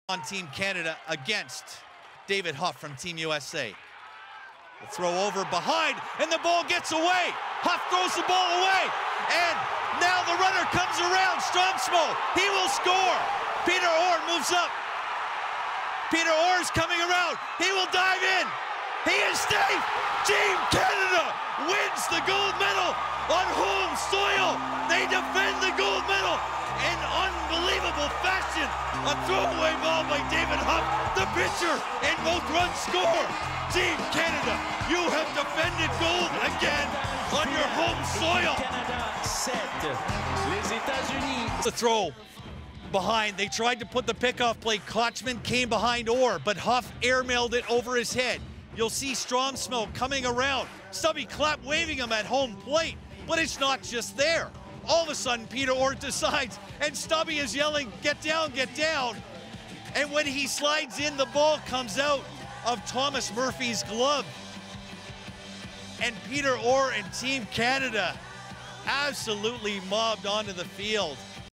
Canada beats U.S. in wild baseball gold-medal game
Botched American pickoff attempt leads to winning run in extra innings. To read the full story